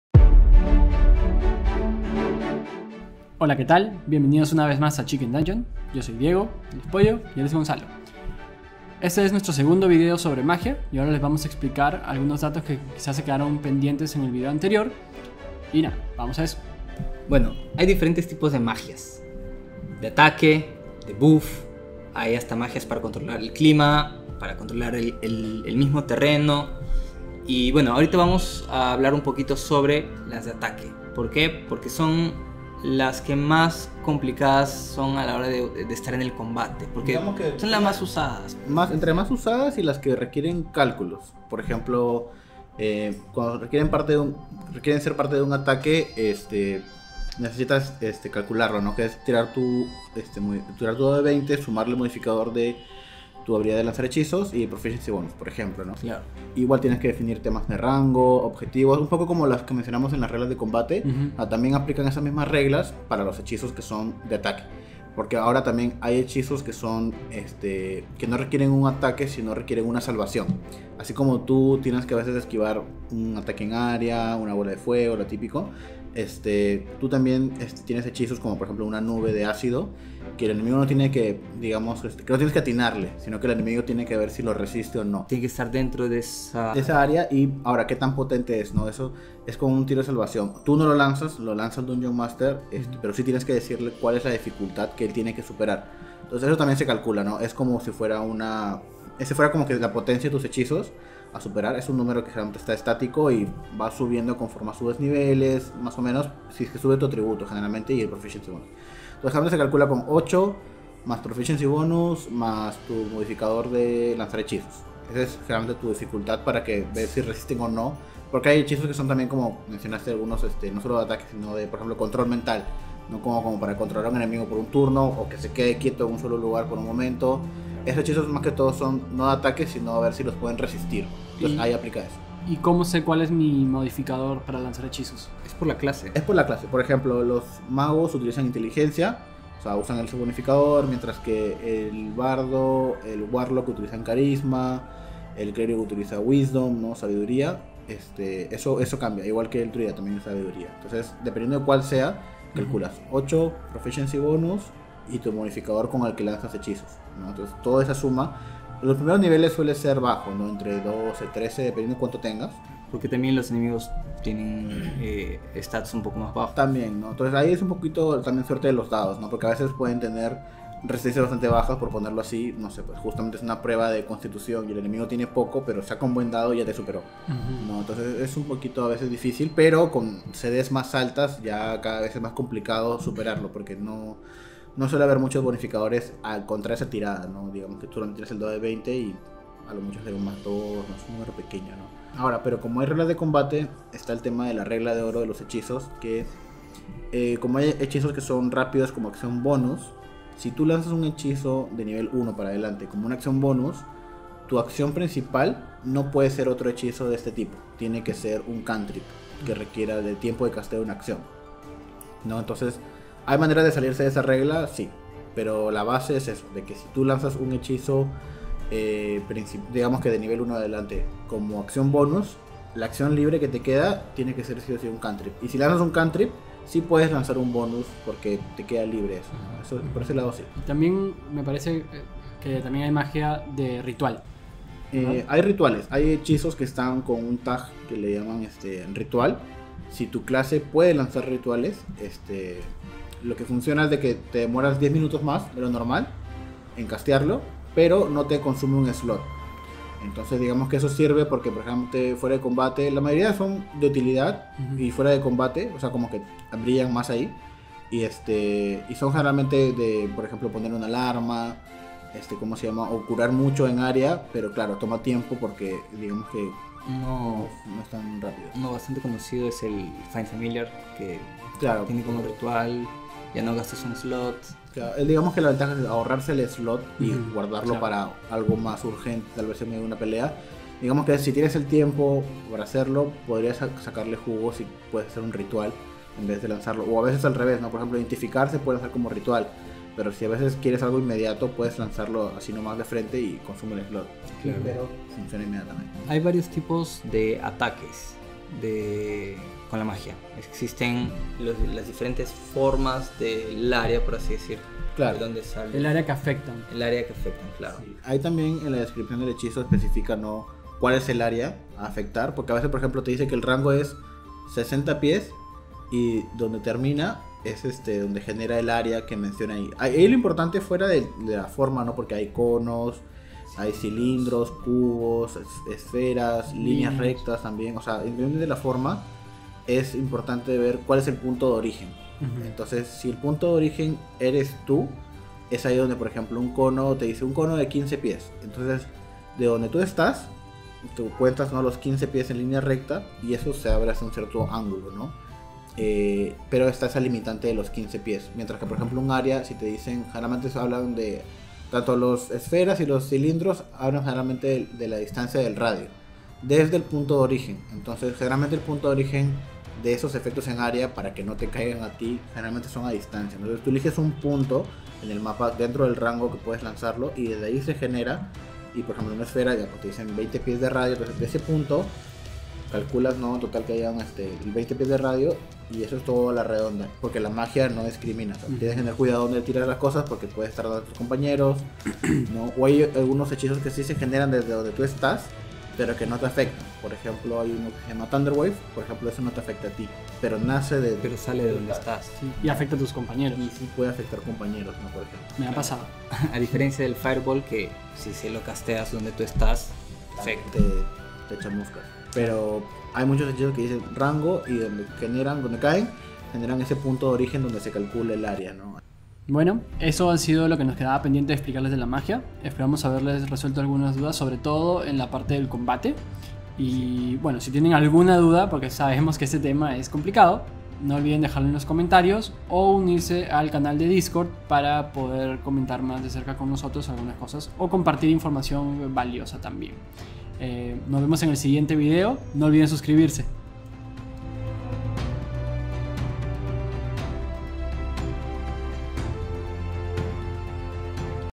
¿Cómo funciona la magia? | Parte 2
La magia es tan variable y puede ser usada para atacar, dar beneficios a tus aliados, debilitar a tu enemigo, entre otras cosas. Además el área de efecto puede ser de diferentes formas.

Aquí les explicamos algunas reglas adicionales sobre la magia, si tienes planeado jugar un lanzador de hechizos, les recomendamos 100% darse un tiempo para leer con calma sobre su clase y sus hechizos disponibles para que escojan con anticipación a la sesión ya que puede  tomar tiempo y la sesión se puede retrasar.

Únete a nuestro canal de Discord donde podrás compartir mapas, música y todo tipo de cosas relacionadas a D&D, también puedes dejarnos consultas.